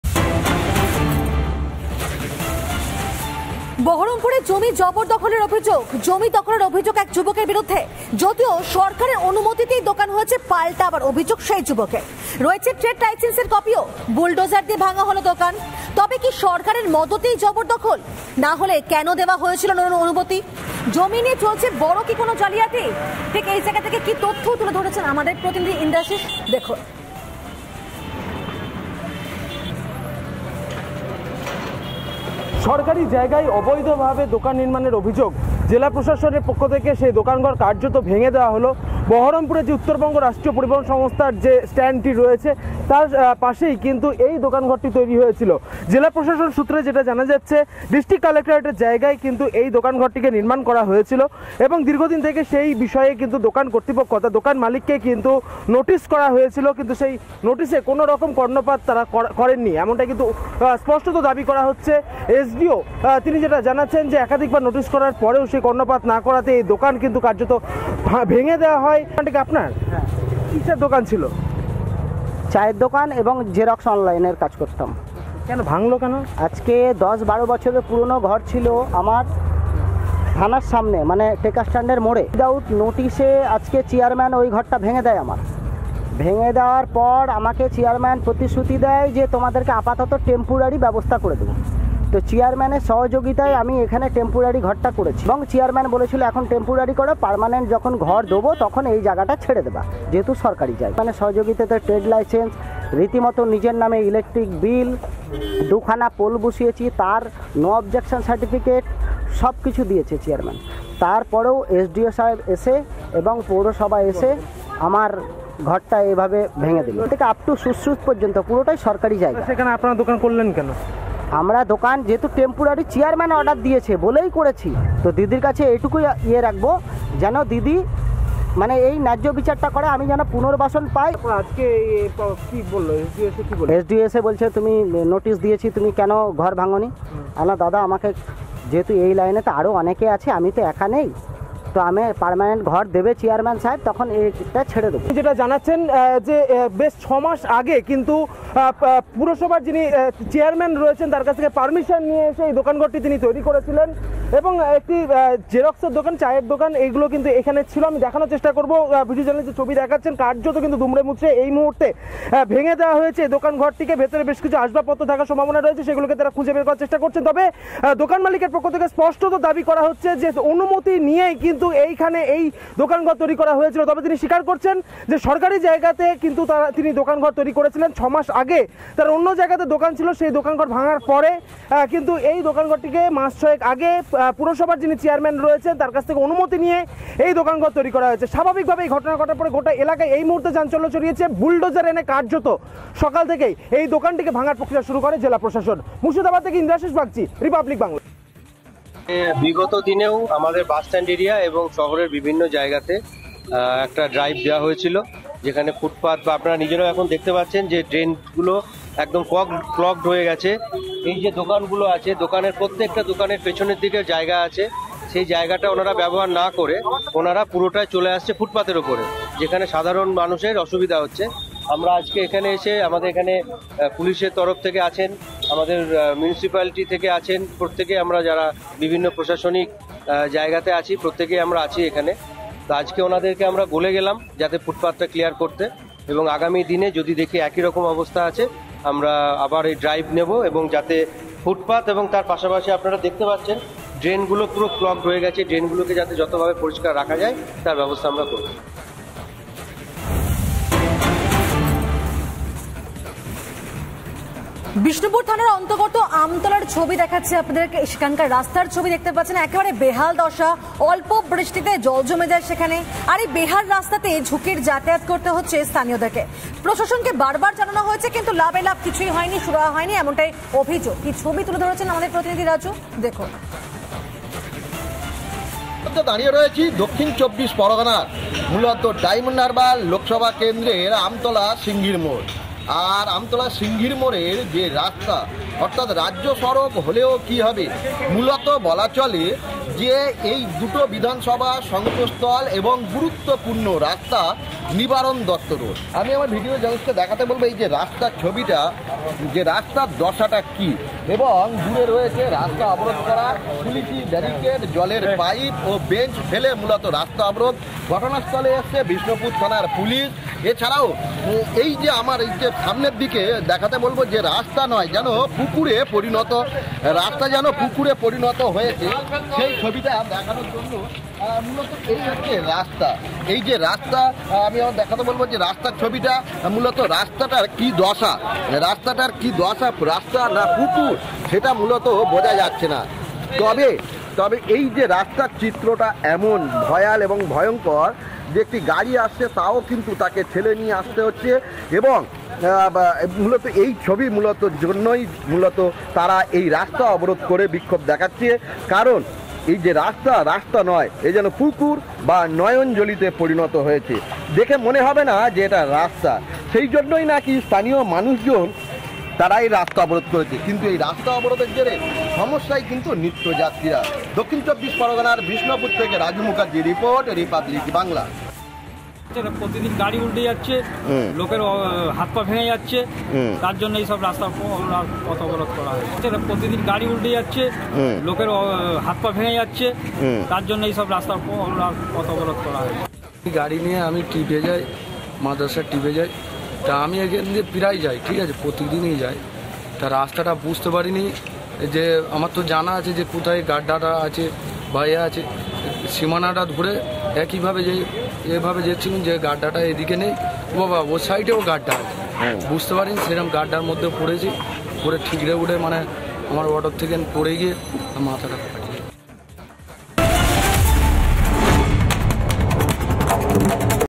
Baharampur News | বহরমপুরে জমি জবরদখলের অভিযোগ। জমি দখলের অভিযোগ এক যুবকের বিরুদ্ধে
Baharampur News | বহরমপুরে জমি জবরদখলের অভিযোগ। জমি দখলের অভিযোগ এক যুবকের বিরুদ্ধে। ‘যদিও সরকারের অনুমতিতেই দোকান’, পাল্টা অভিযোগ যুবকের

রিপাবলিক স্বাধীন।
রিপাবলিক নির্ভীক।
আপনিই রিপাবলিক।